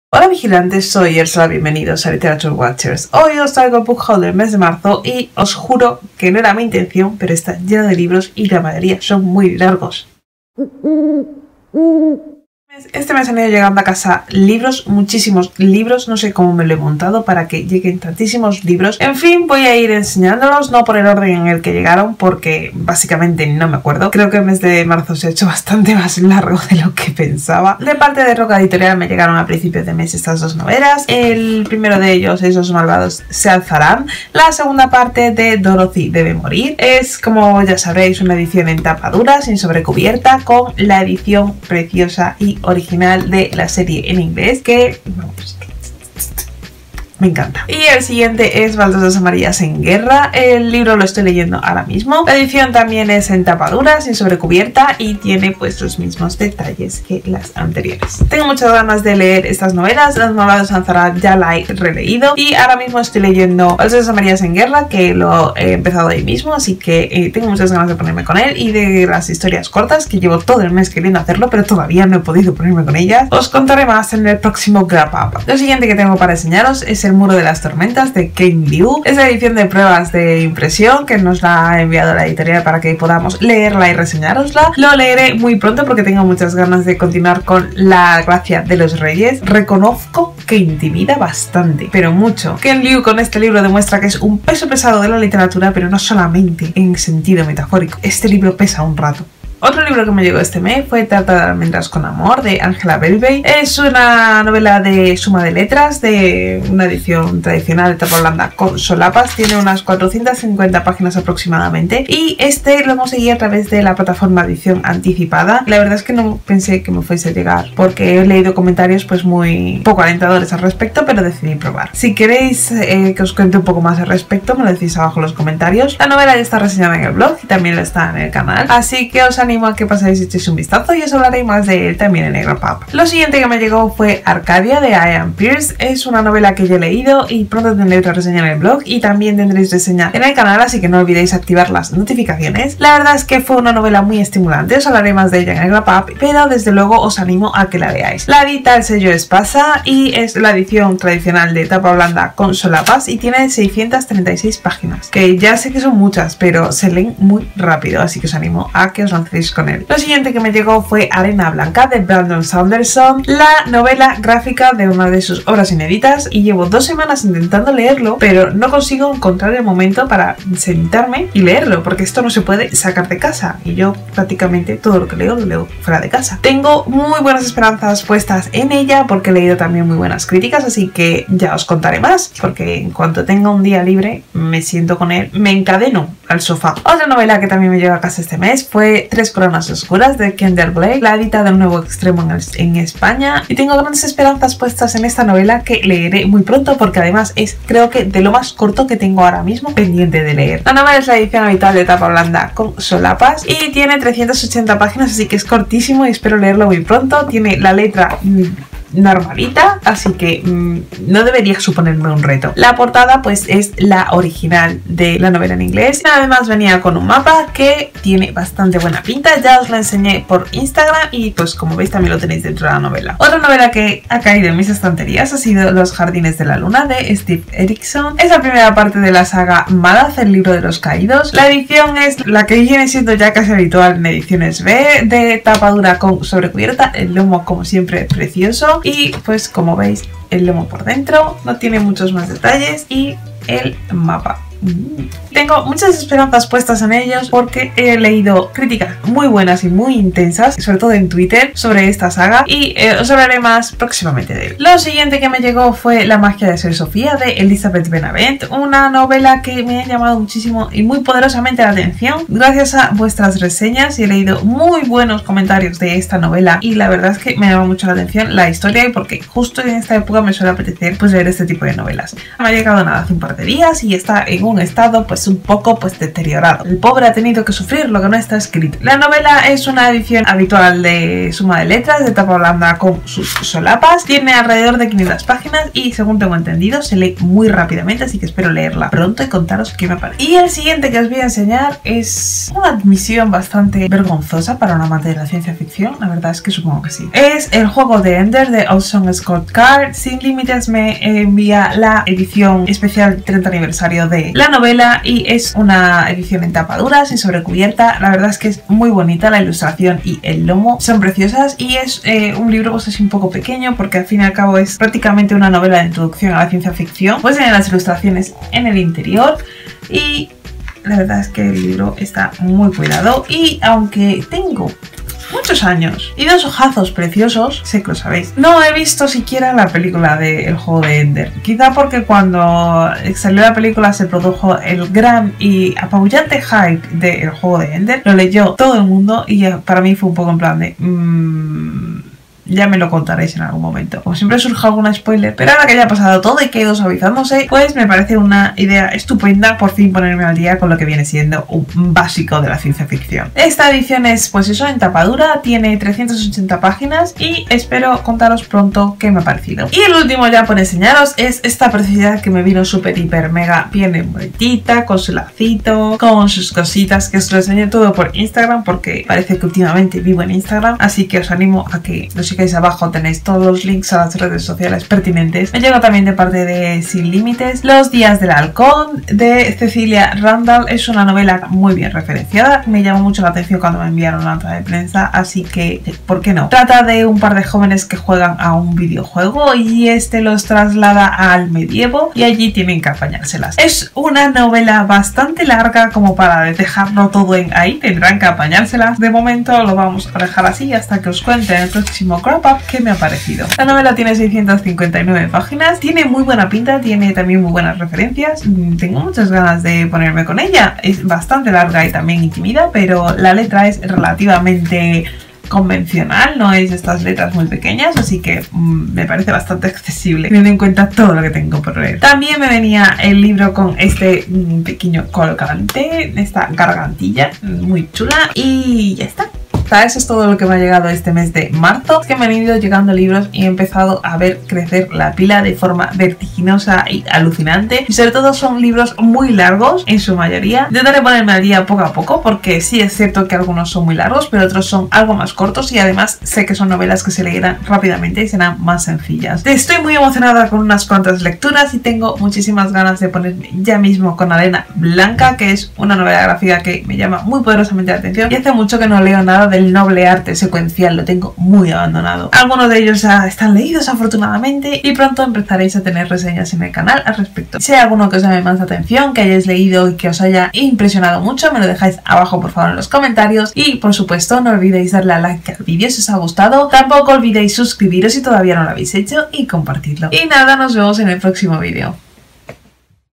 Book Haul Marzo | LitWatchers
Hola vigilante,
¡Bienvenido a un nuevo vídeo!
En esta ocasión os hablo de las novedades que han ido llegando a mis estanterías durante el mes de marzo, con ediciones ingeniosas y muy bonitas que os conquistarán.